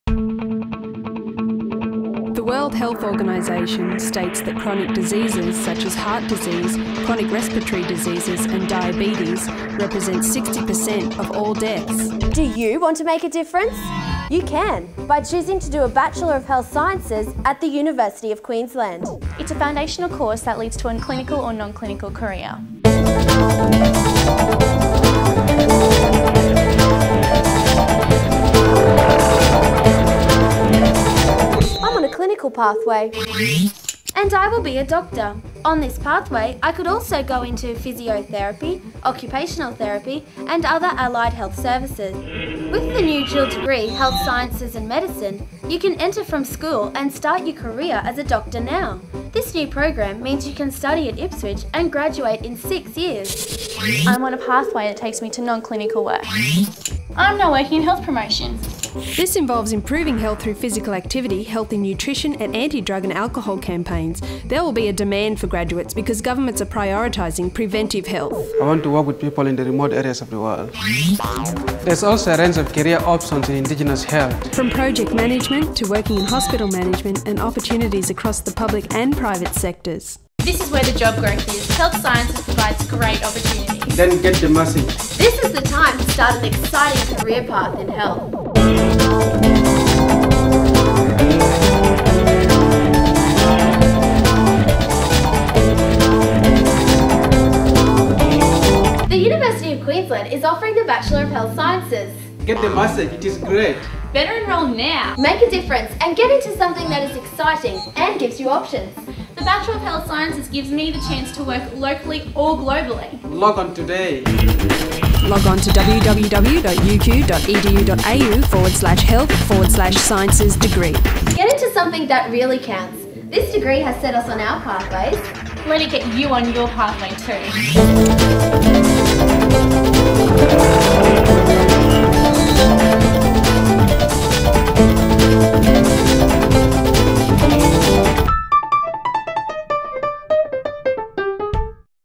Bachelor of Health Sciences
A video about Health Science and studying at the University of Queensland